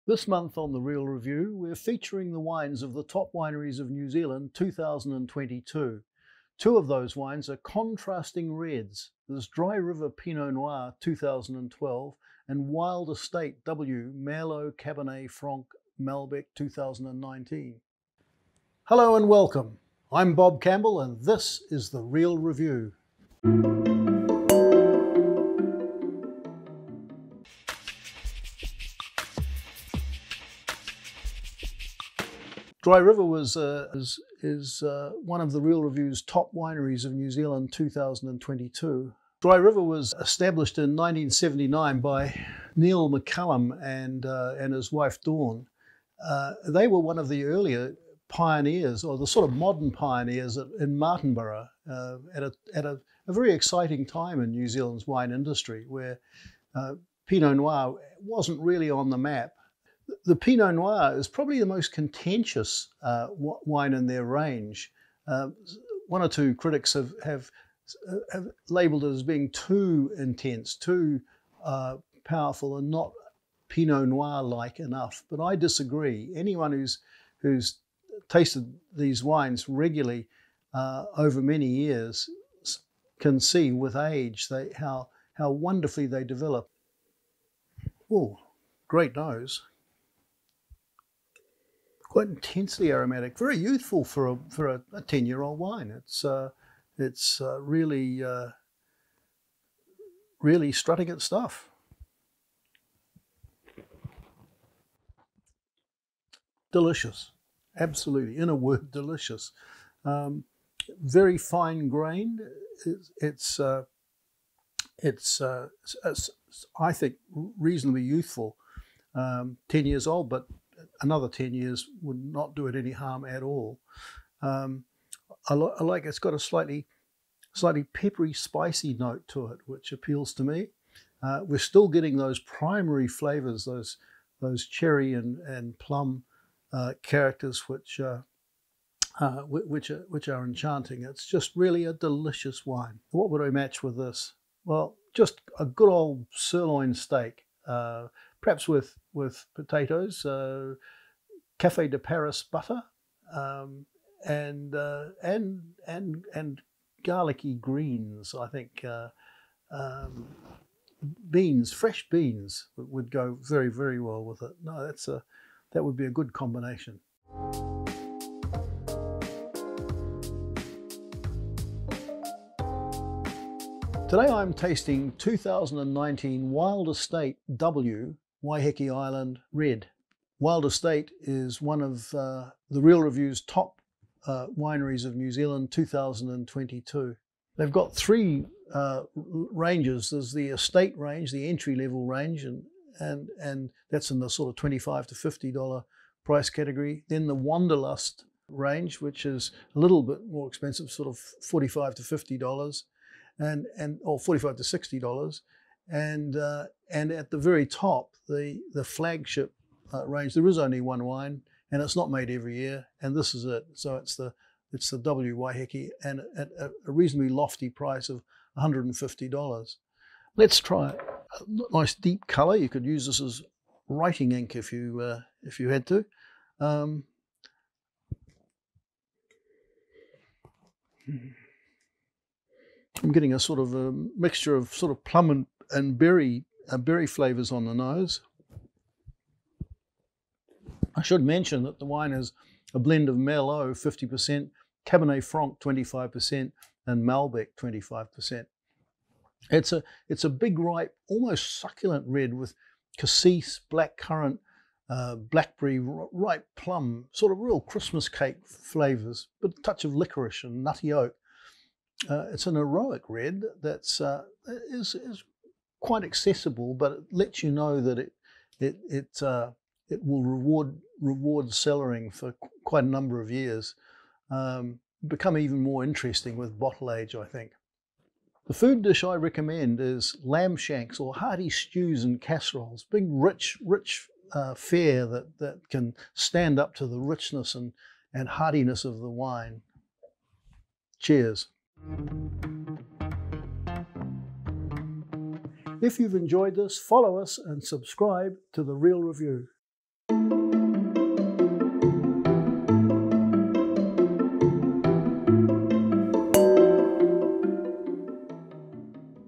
Top Wineries of New Zealand 2022: Two contrasting reds
This month at The Real Review we are celebrating the Top Wineries of New Zealand 2022. Today, we're tasting two contrasting reds: a pinot noir and a Bordeaux blend.

Matched with steak frites with Cafe de Paris butter

Come and taste the Top Wineries of New Zealand 2022
🍷 Auckland, Oct 9